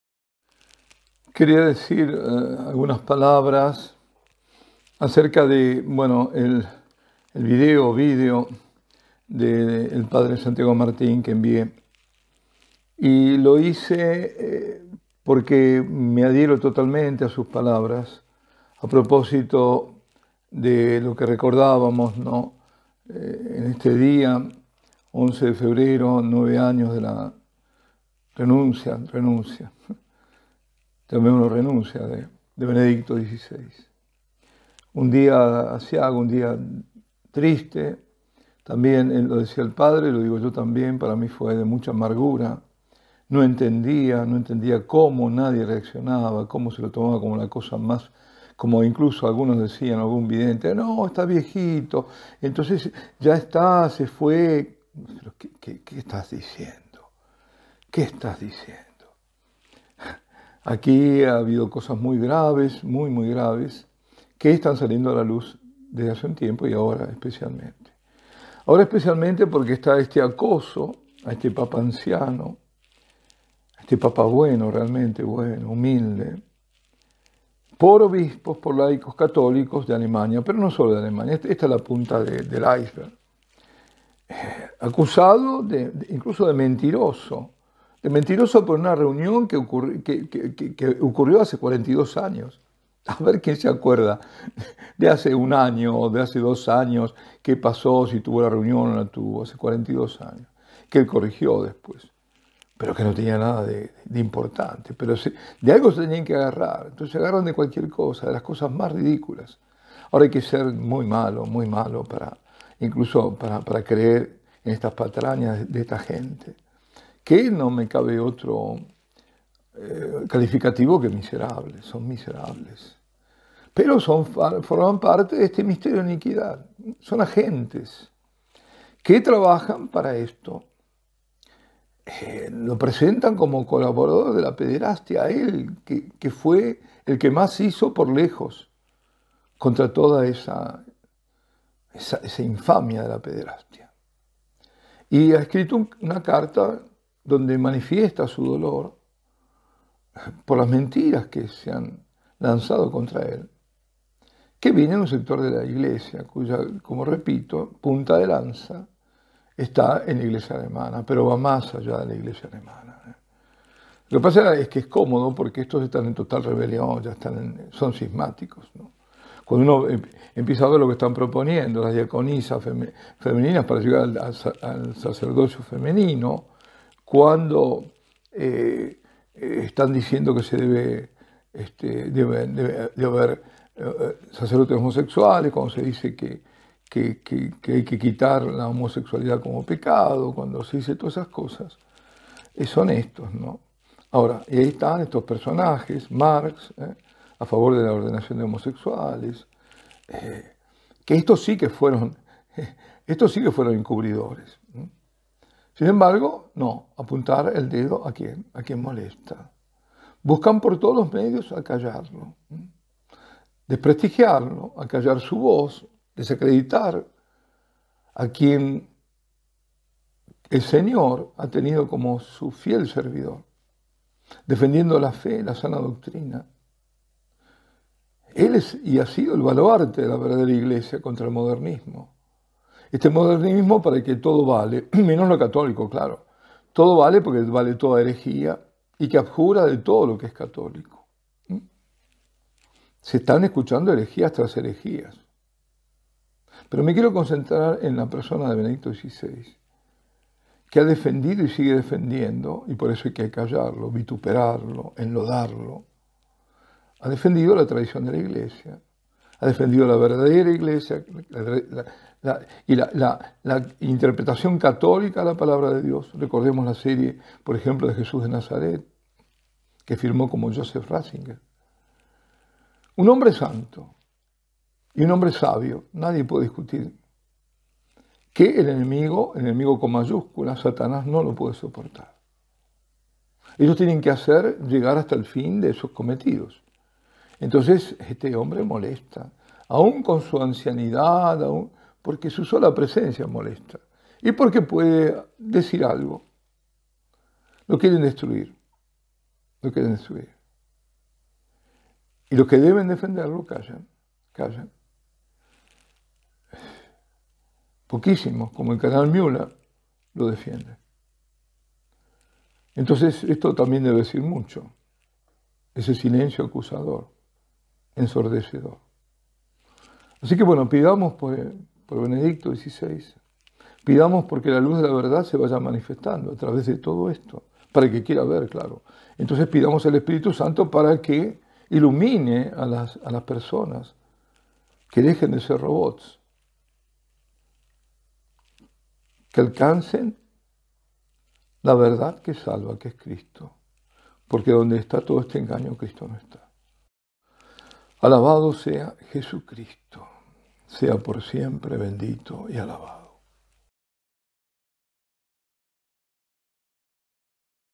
Unas palabras sobre la infamia desatada. P. Justo Antonio Lofeudo.
En defensa del Papa Benedicto XVI ante las infamias desatadas.